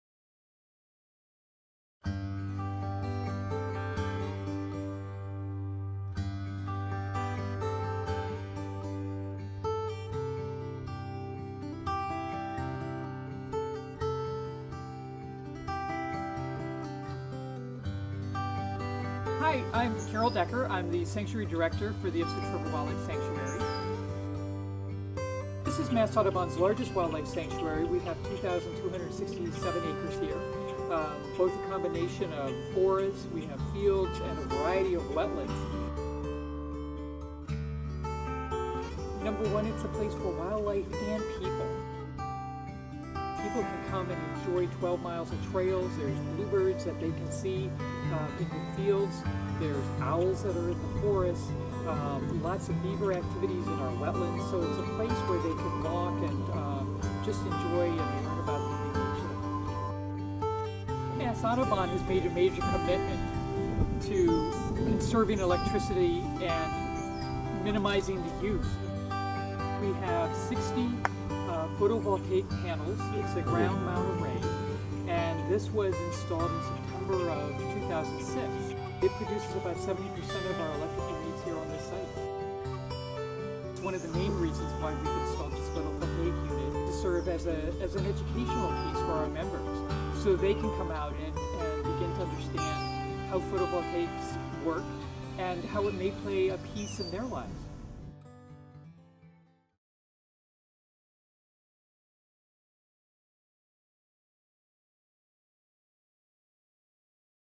Green Technology at Ipswich River Wildlife Sanctuary
Overview of the green technology features of Mass Audubon's Ipswich River Wildlife Sanctuary in Topsfield, Massachusetts. Video produced by Cam Boll.